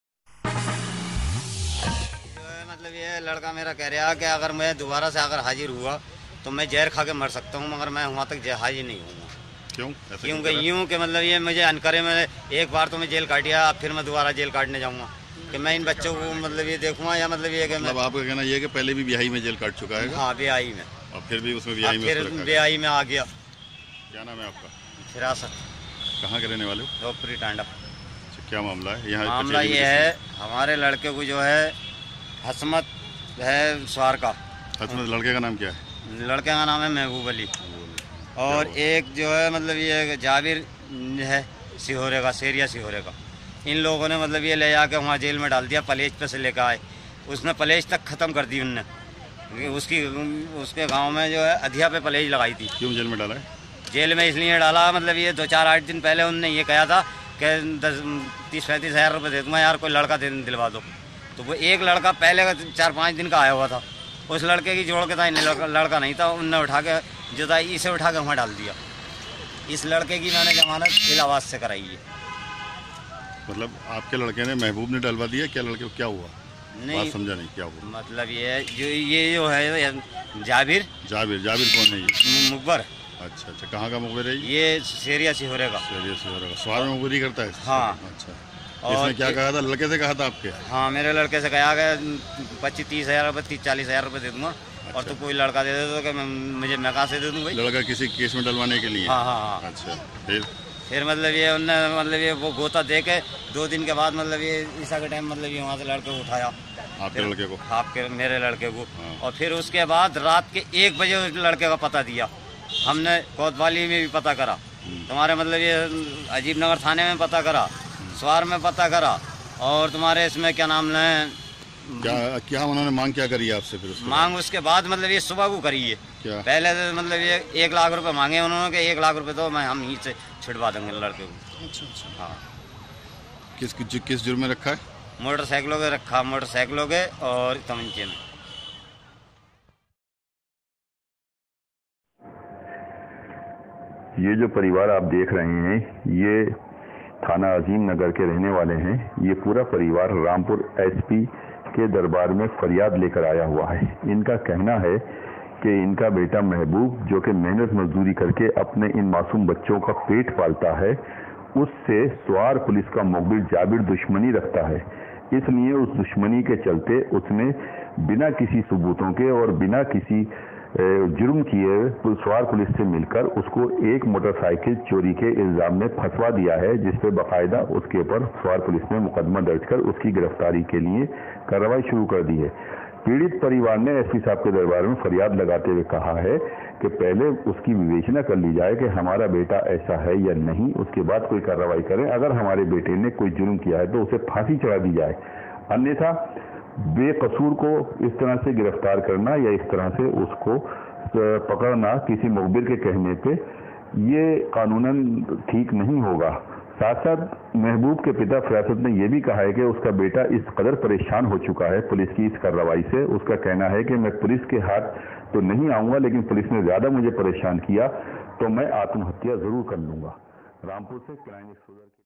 Suwar पुलिस पर लगे उत्पीरण के आरोप है
बेक़सूर को बाइक चोरी में फ़साने का आरोप